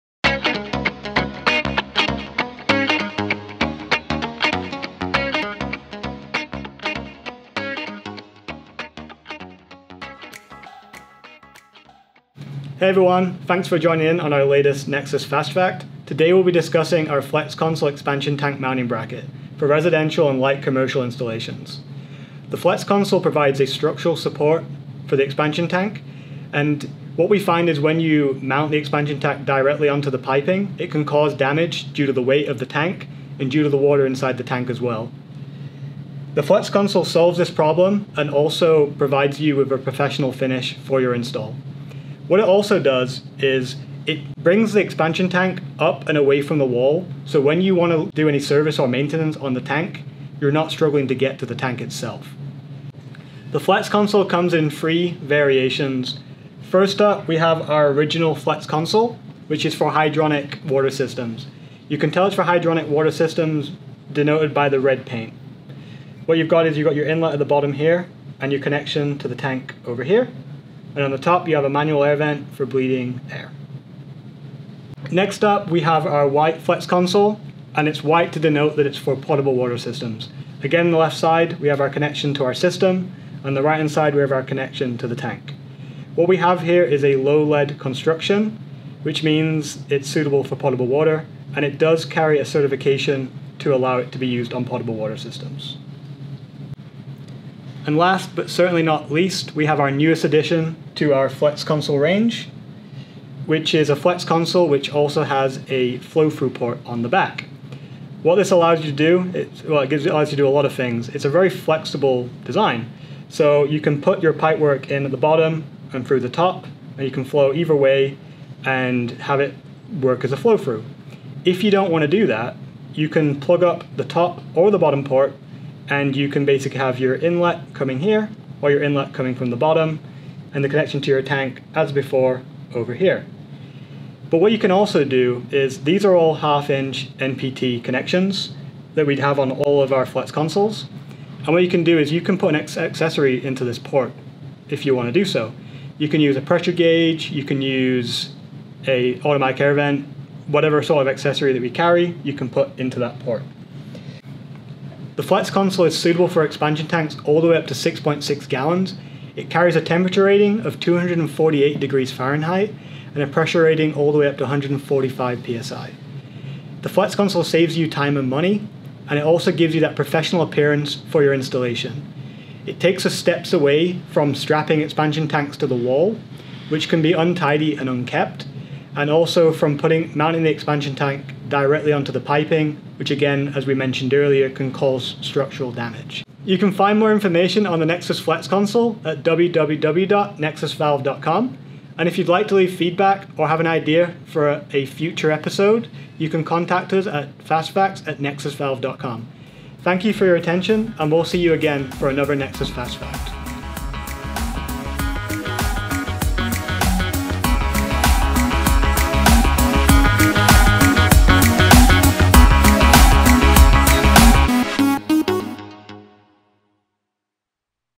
Nexus Fast Facts: Flexconsole
The Nexus Flexconsole easily mounts expansion tanks securely to the wall and can hold up to 6-gallon tanks. We provide 3 different models, including one that is WQA certified 371/62 for potable water installations.